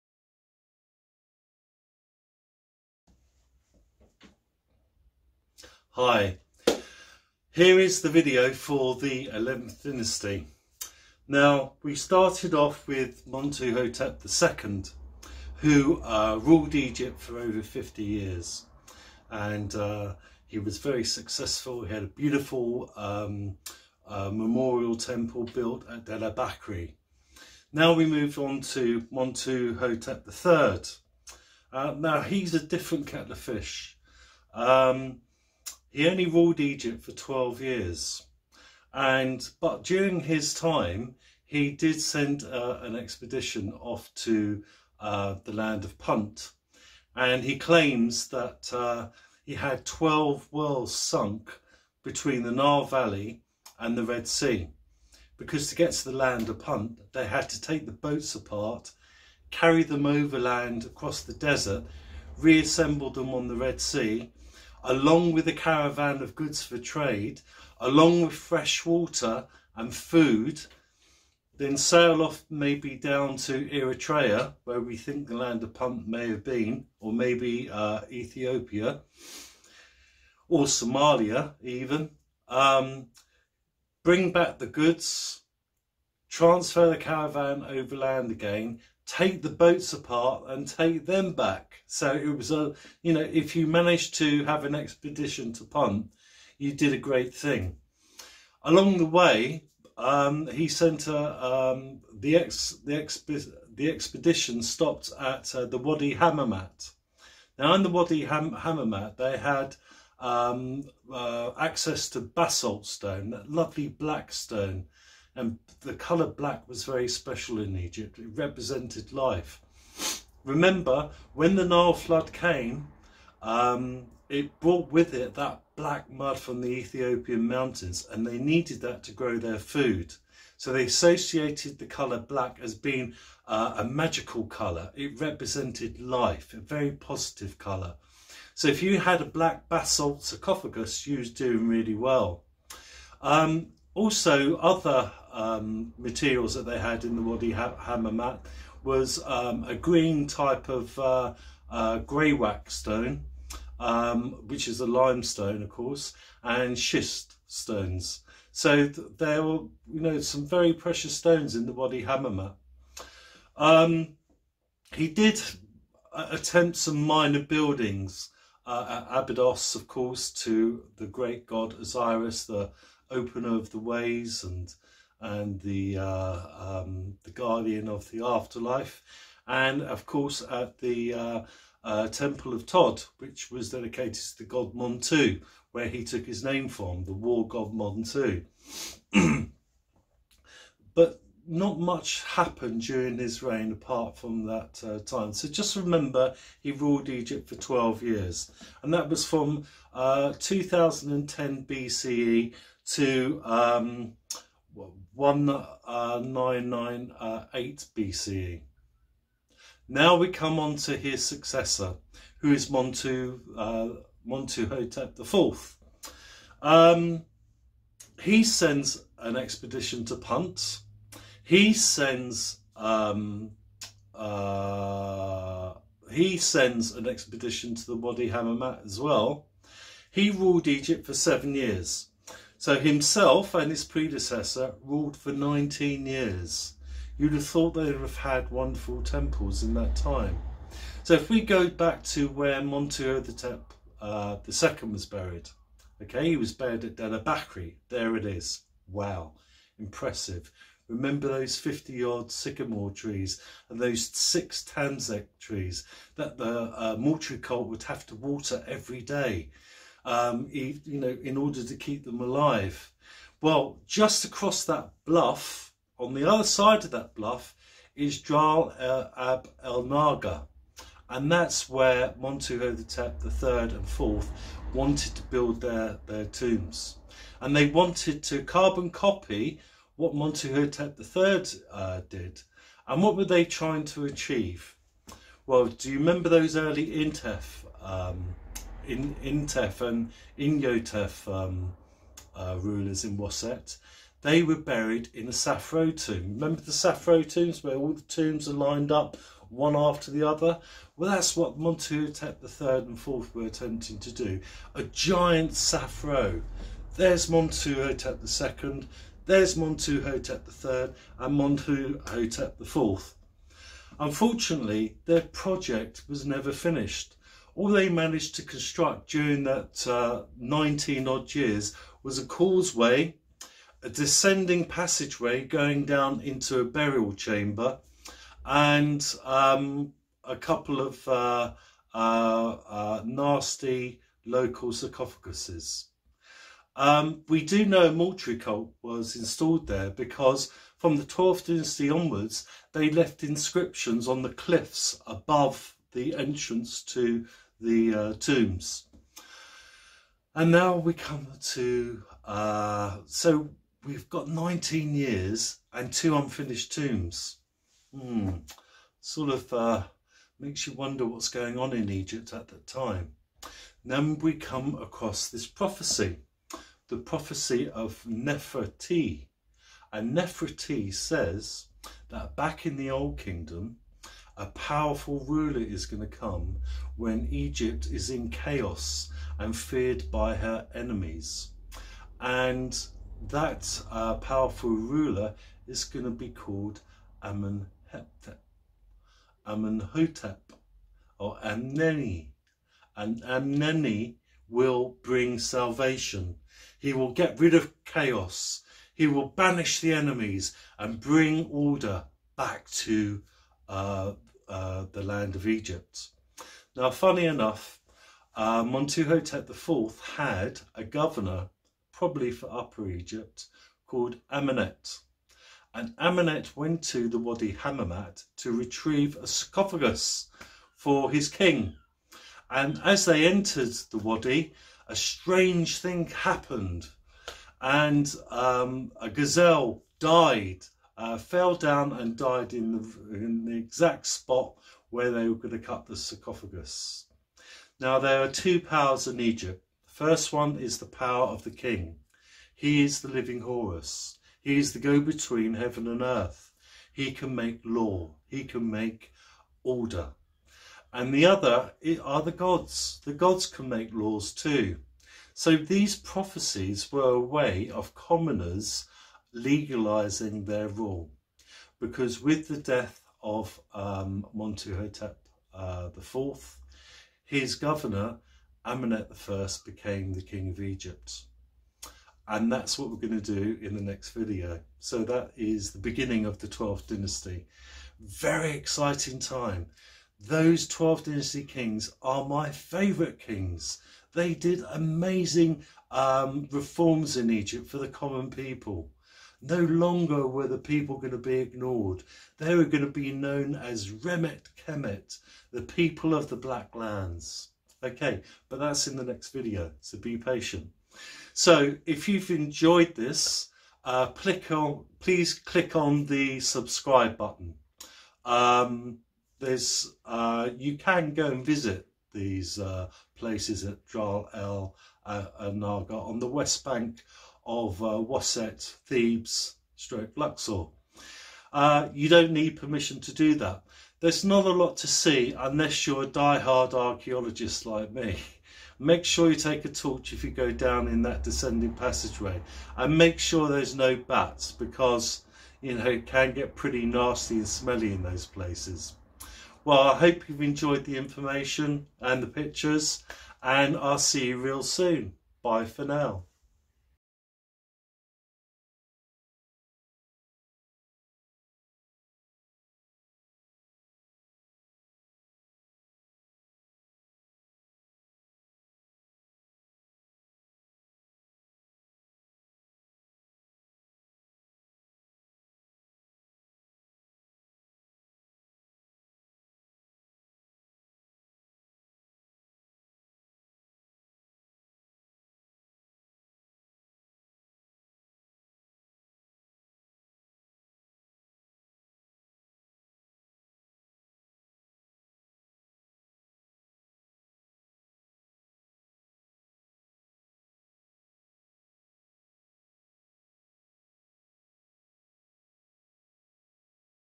XI Dynasty, Ancient Egypt, North Africa
A look at the kings, Monuthotep III and Montuhotep IV